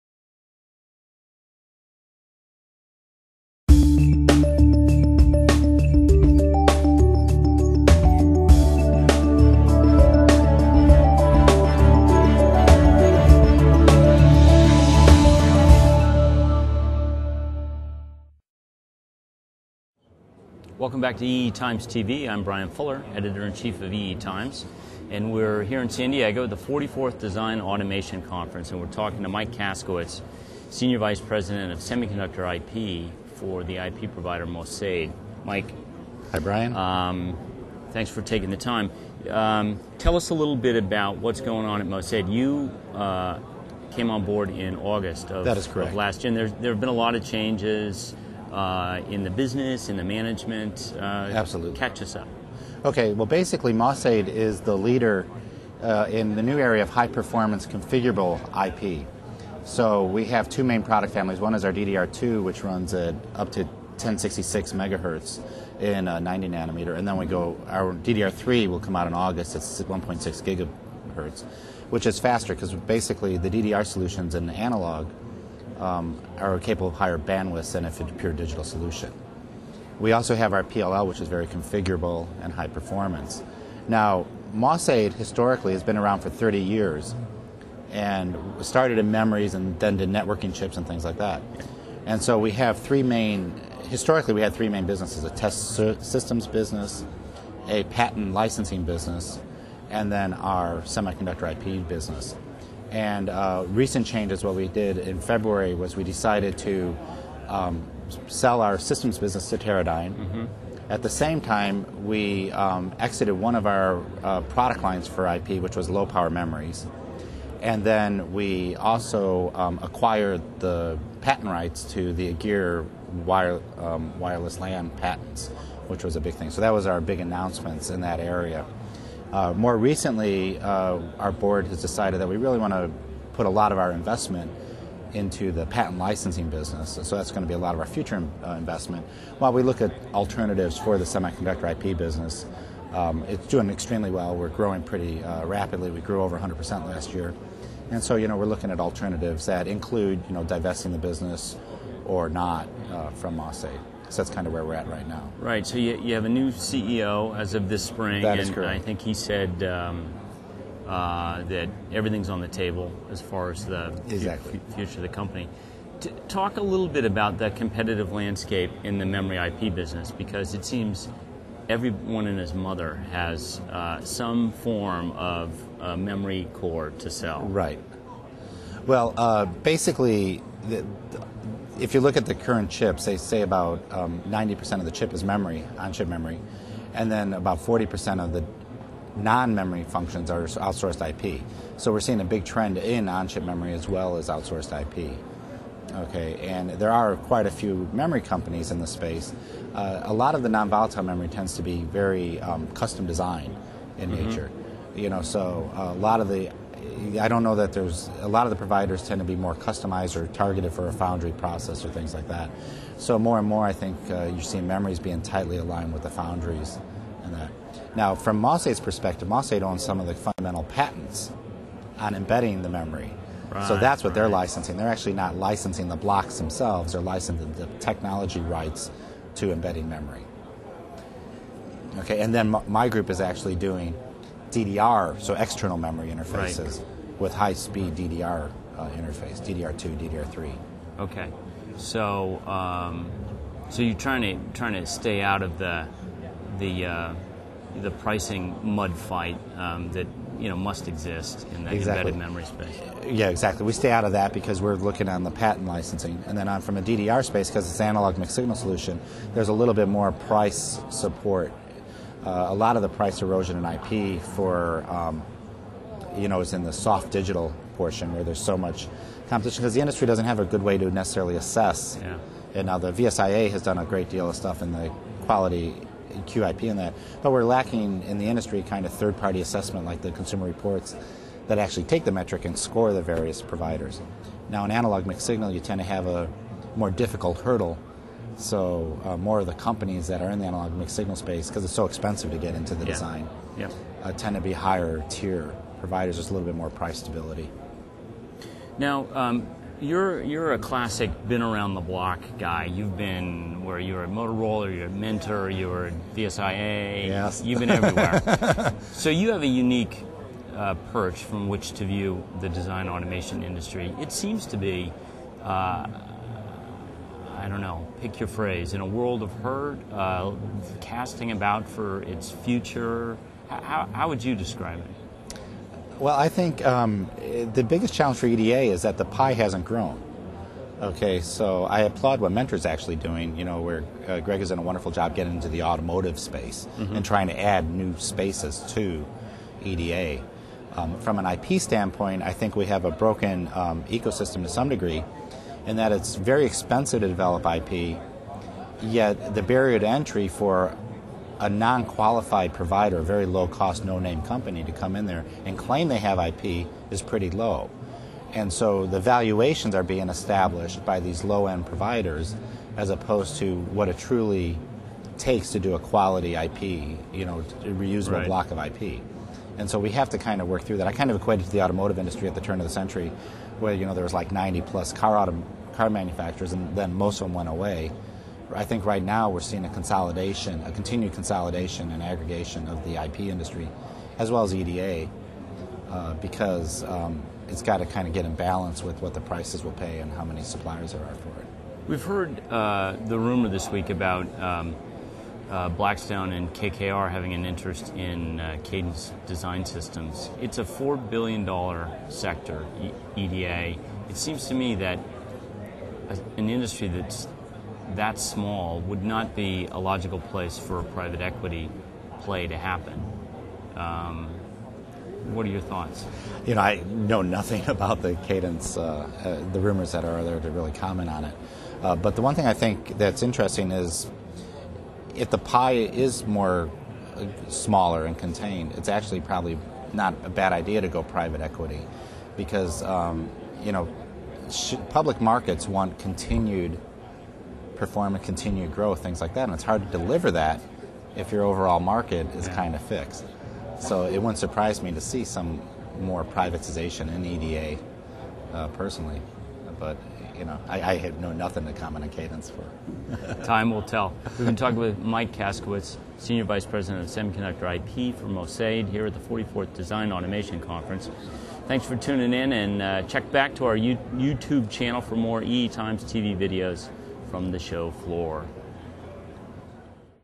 Mosaid's Kaskowitz on Happenings at Company
Michael Kaskowitz, Senior VP Semiconductor IP talks with Brian Fuller of EE Times about the company and the future